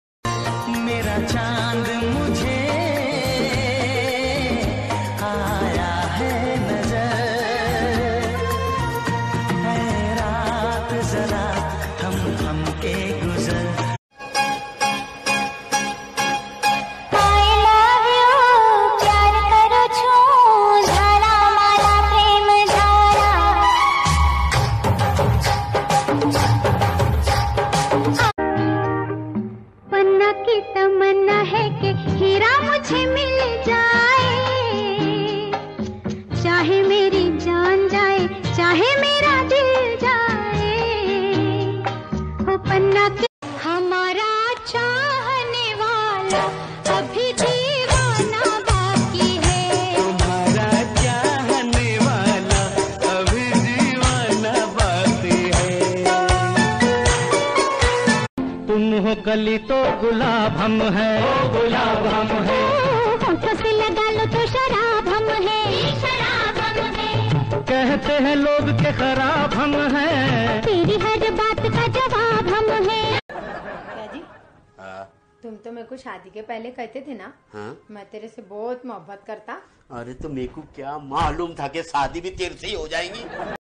Pyar ishq muhabbat or phir takrar || romantic video || recommended || watch video || Rashid khan ...
Phalay pyar phir ishq us k bad muhabbat is k bad to takrar bhi deakhia is video main .